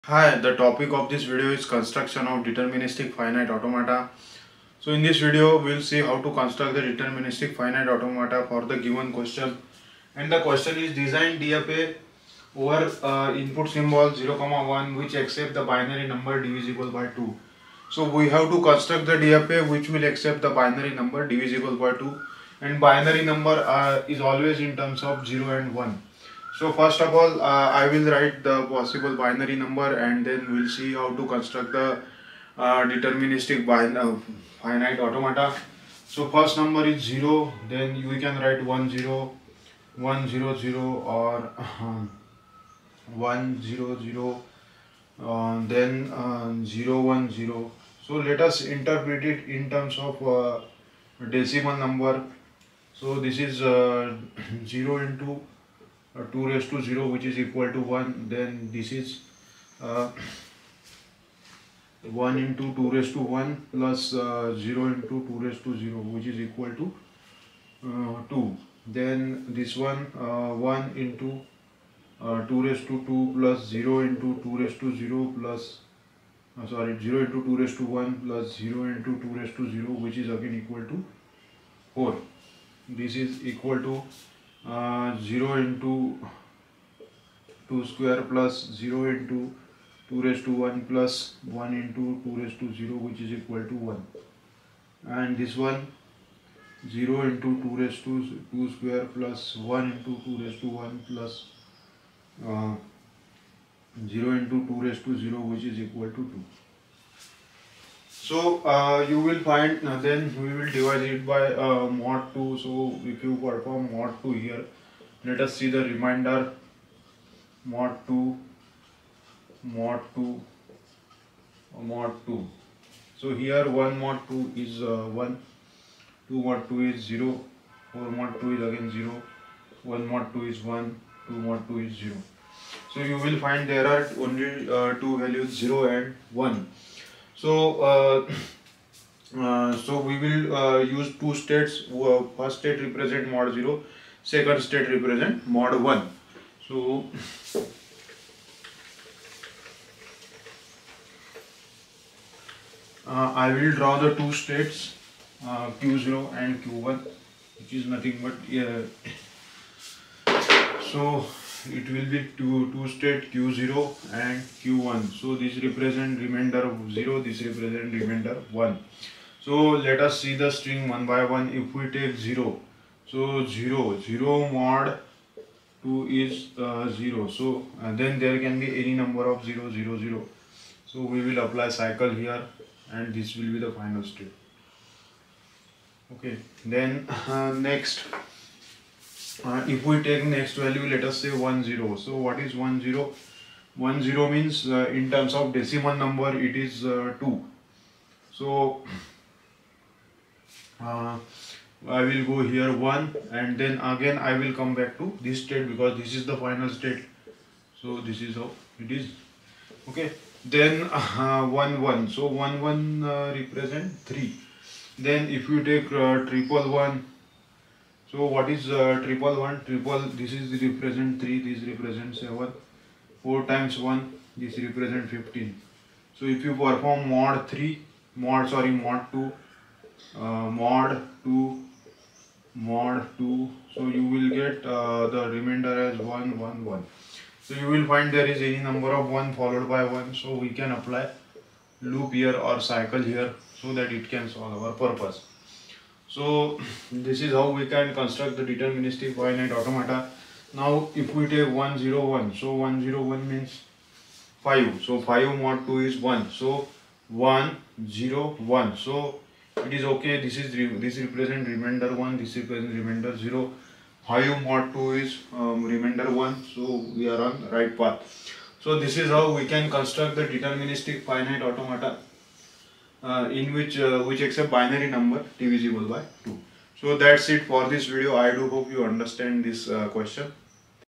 DFA Accepts Binary Number Divisible by 2 | Theory of Computation | Formal Language Automata Theory
This is Dr. (Prof.) Ashish Chandak | The aim and Objective of Quick Engineering Channel is to deliver quality content in short duration.